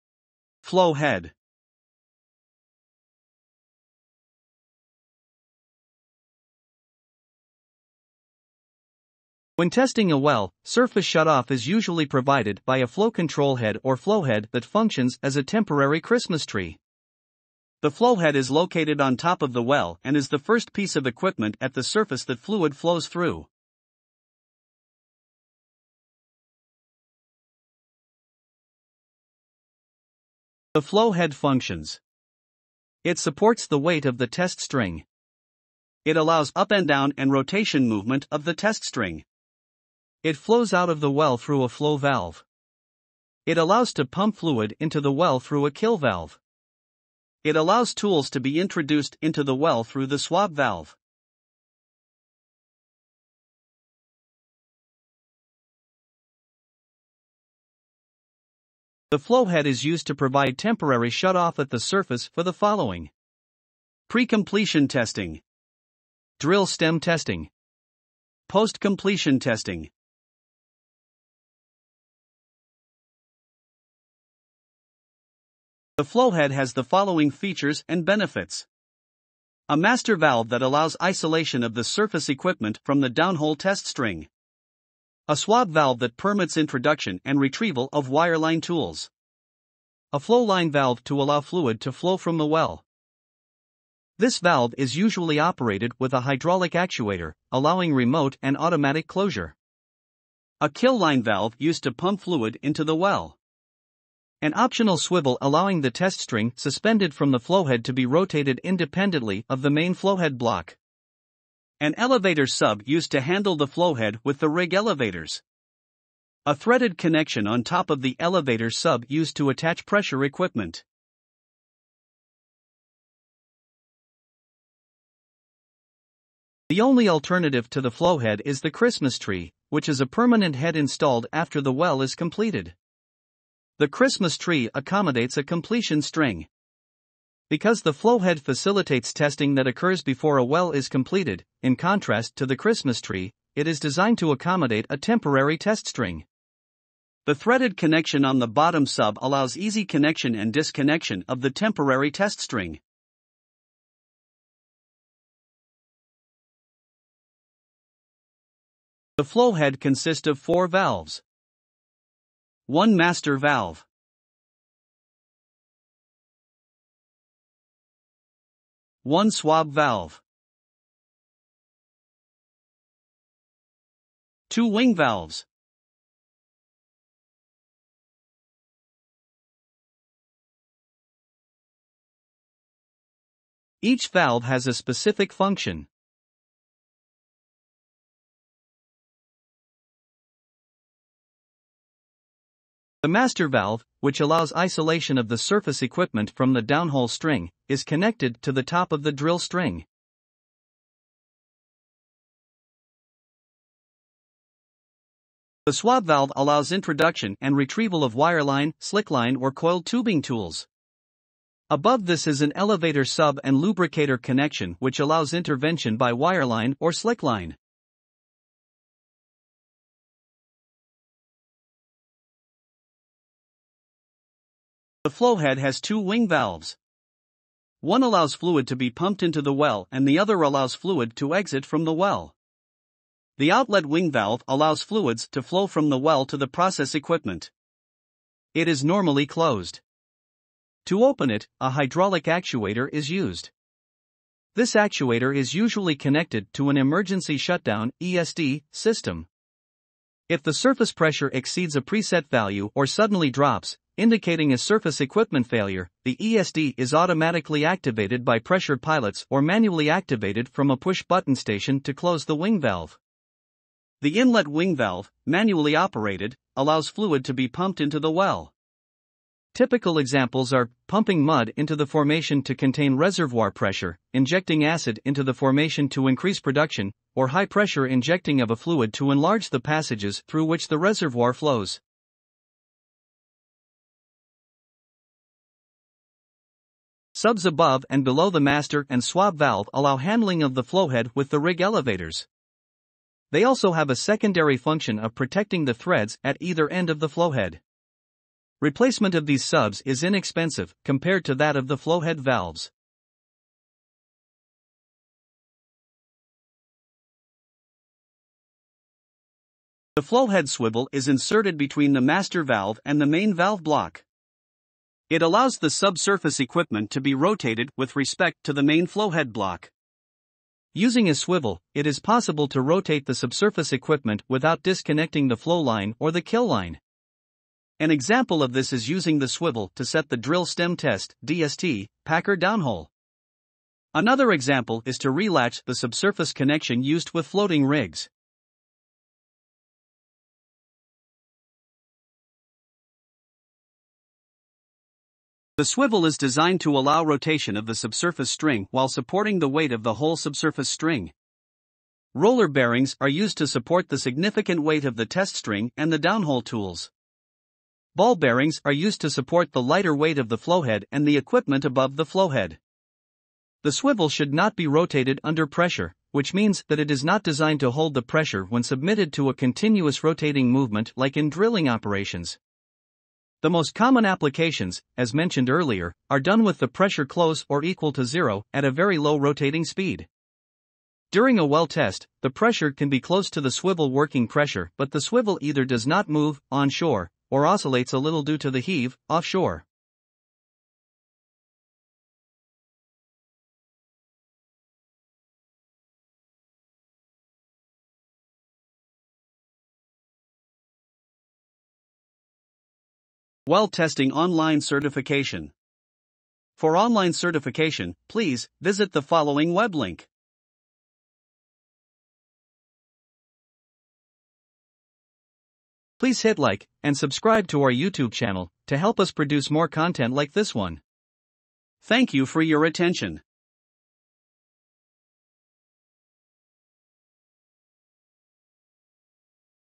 SWT - Flowhead
The Flowhead is the first piece of equipment on a surface well testing package that handles flow at surface.